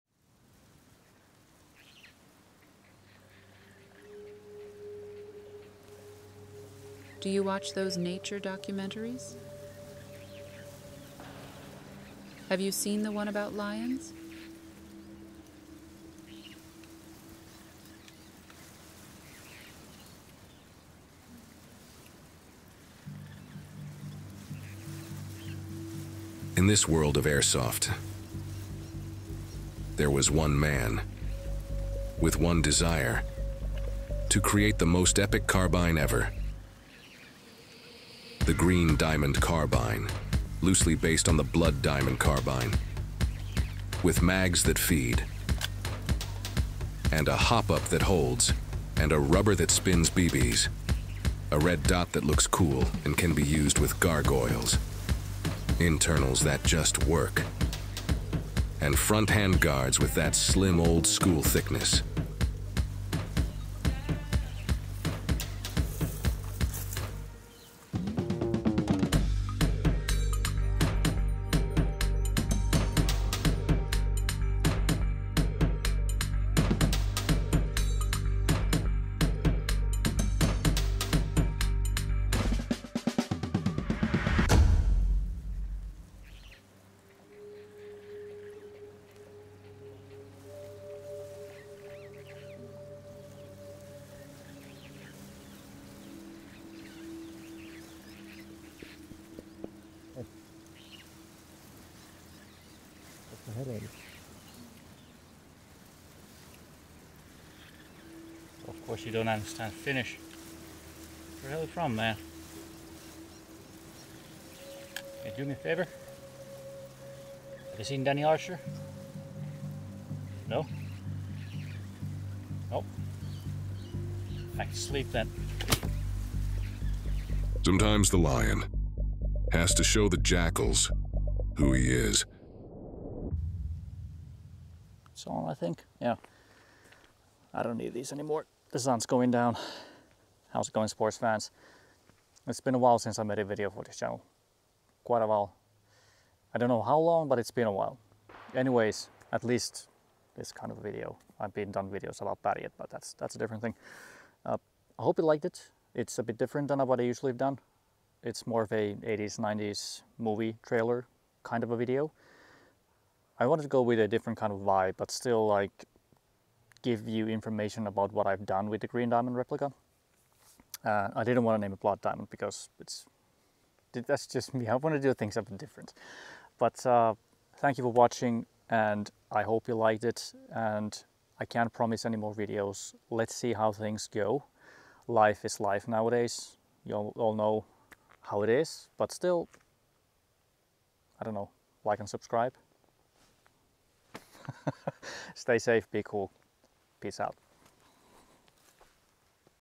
Short 'n' Sweet - The Green Diamond Carbine
A short breakdown of the Green Diamond Carbine airsoft replica that I put together. The base airsoft replica was a Cyma CM009F.

Big thanks to Milgear for their support in building this replica.
You can also check out their YouTube channel: @milgear9394